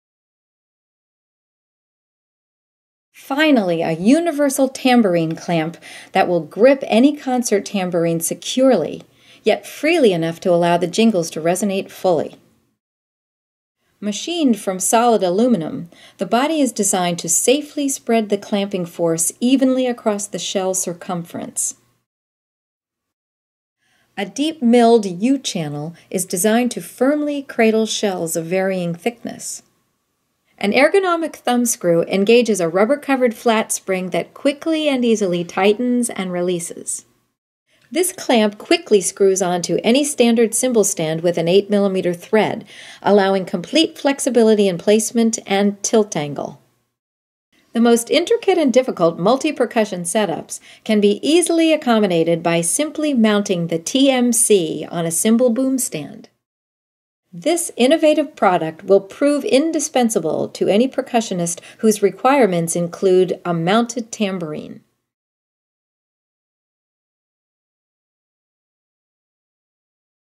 Grover Pro Tambourine Mounting Clamp
Finally, a sturdy universal tambourine clamp that will grip your 8-, 10-, or 12-inch Grover tambourine securely, yet freely enough to allow the frame and jingles to resonate fully!
Machined from 6061 aircraft-grade aluminum, our clamp is designed to spread the clamping force evenly across a wide 4-inch length of the shell circumference. This assures that no damage will occur to your instrument, even if played vigorously. The ergonomic thumbscrew engages a rubber-covered flat spring that quickly and easily tightens and releases. The deep-milled U-channel is designed to securely and safely cradle ANY model Grover tambourine.
One of the best features of our versatile mounting clamp is ease of use; it quickly screws onto any standard cymbal stand with an 8mm thread. This allows complete flexibility in placement and tilt angle. By mounting on a cymbal boom stand, you can increase the reach to accommodate the most intricate multi-percussion setups.